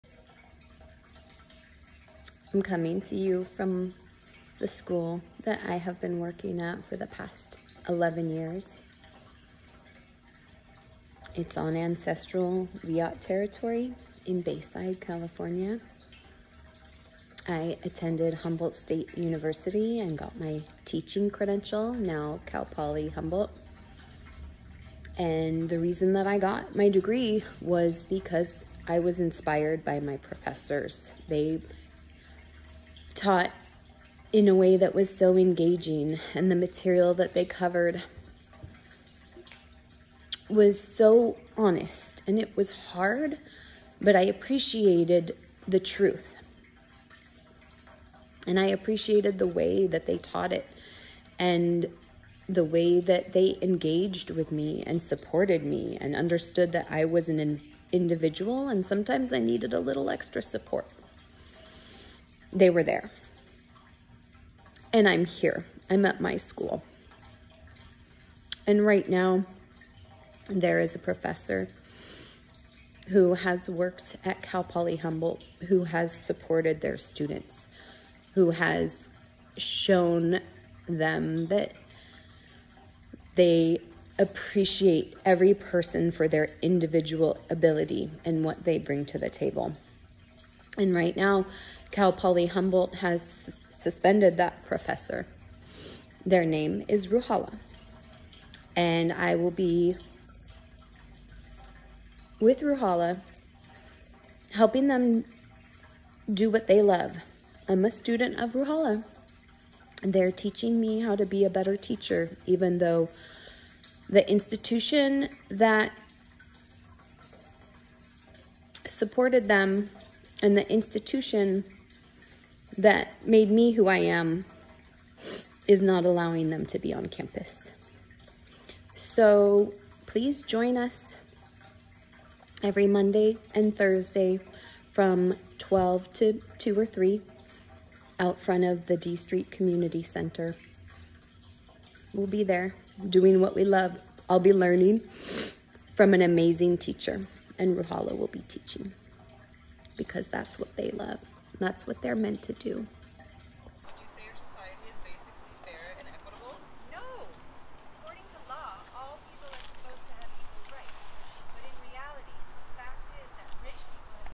Teaching in EXILE: community learns from suspended professor
A local teacher makes a statement in support of professor suspended by Cal Poly University as investigation into their role during campus protests continues, barring the professor from teaching on campus, or communicating with students on campus.